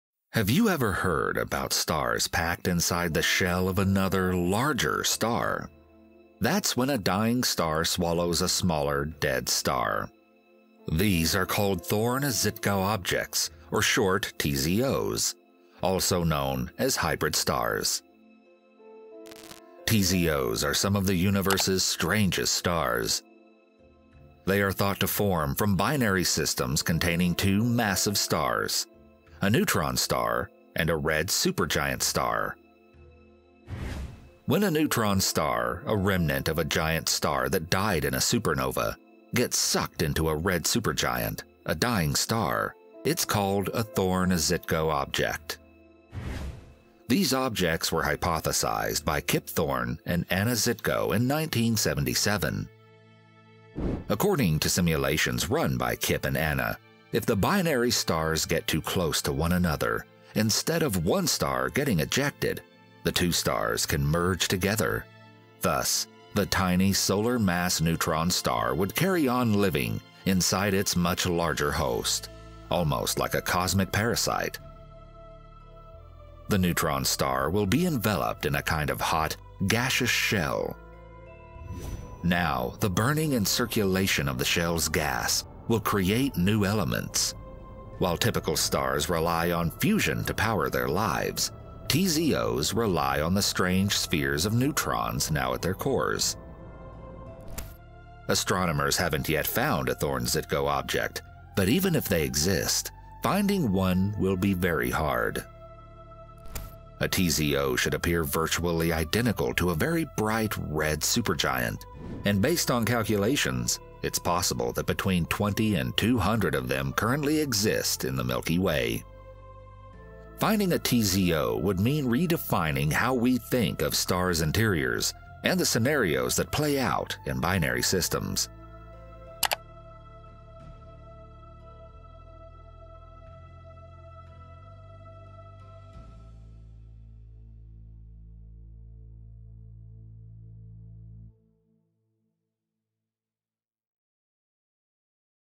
Thorne–Żytkow object: A Star Within A Star
When a red supergiant star swallows a dead star it's called a Thorne-Żytkow object. These objects were hypothesized by Kip Thorne and Anna Żytkow in 1977.

Edited by:
Ardit Bicaj

Narrated by:
Russell Archey

Graphics:
Space Engine

Thank you to our Patrons:
DonPig
Joe Chappell